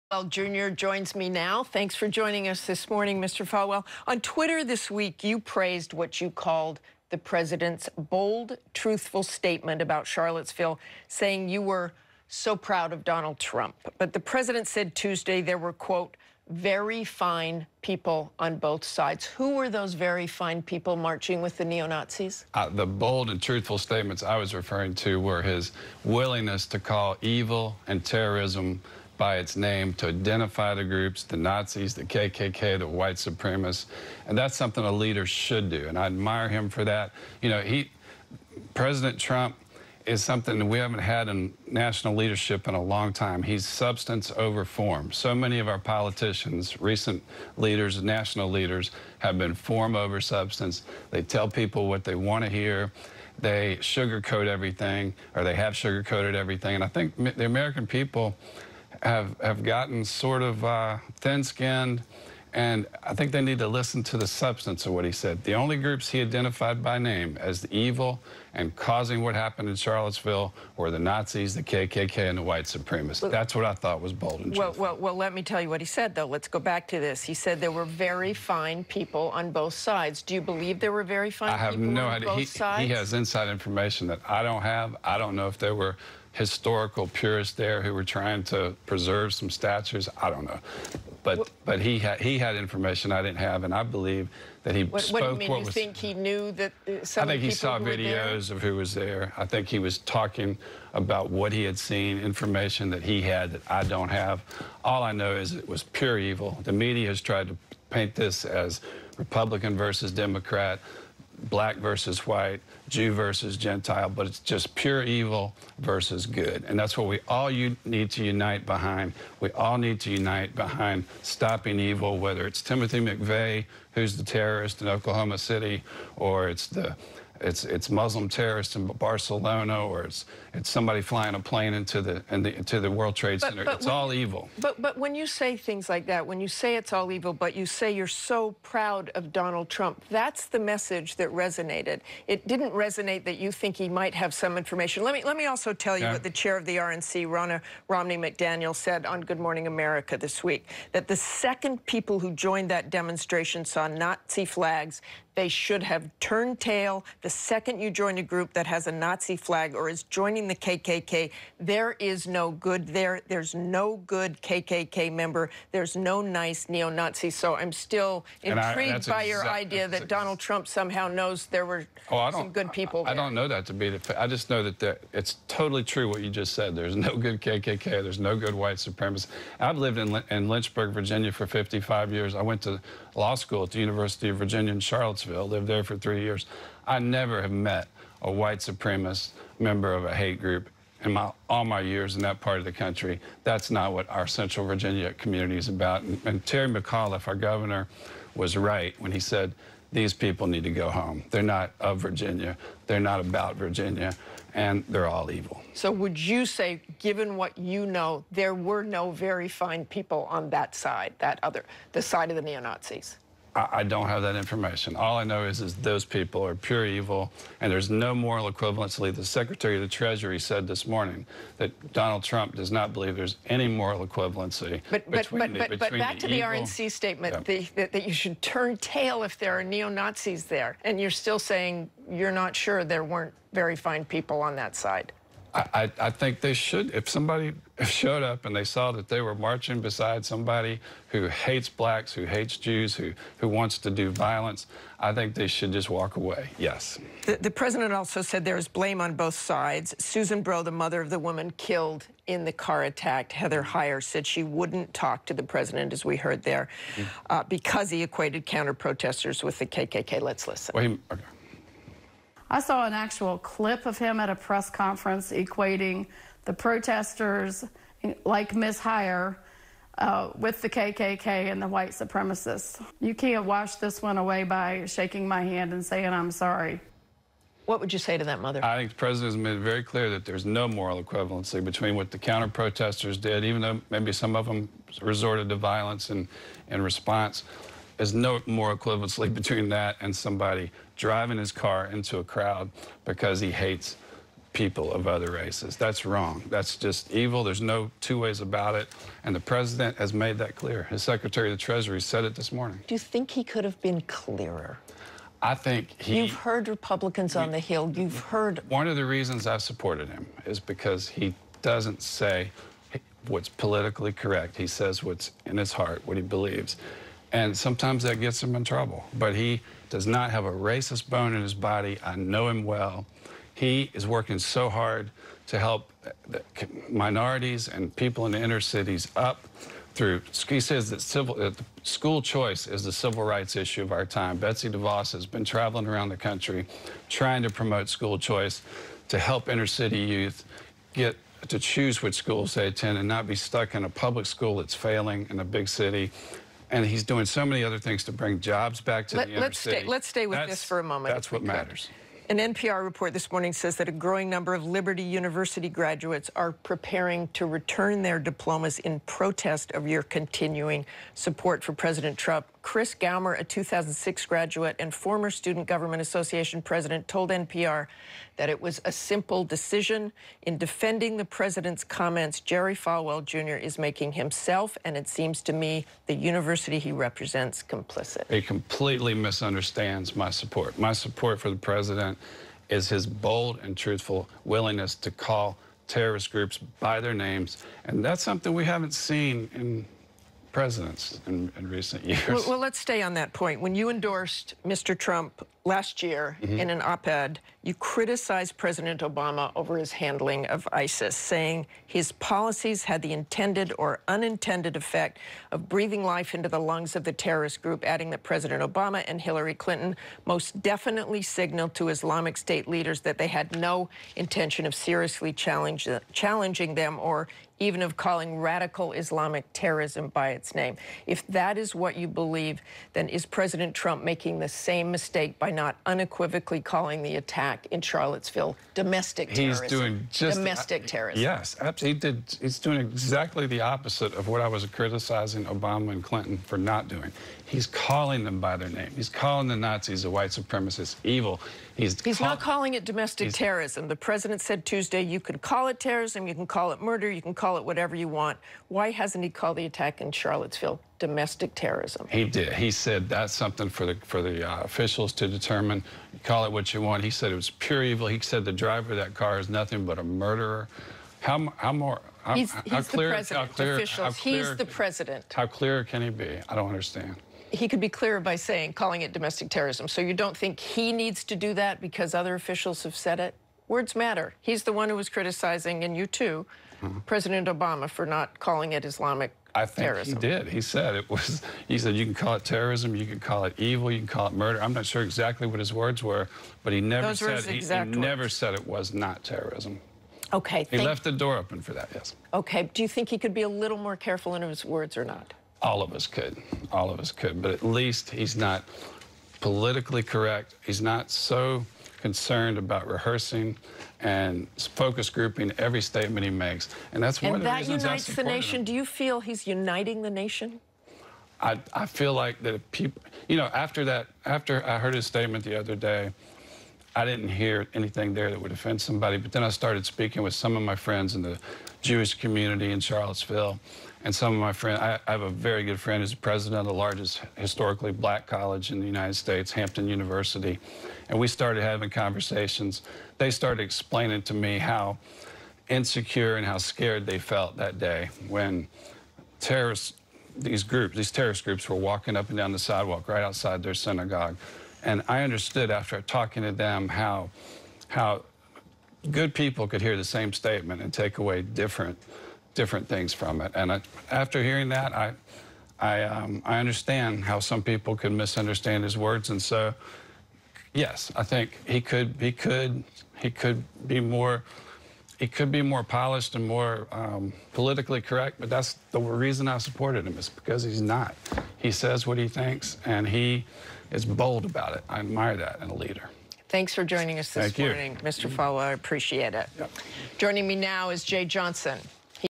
Falwell Jr. defends Trump’s racist presser: He has secret videos showing ‘fine people’ marching
Learn more about news on Youtube: goo.gl/pYLSVe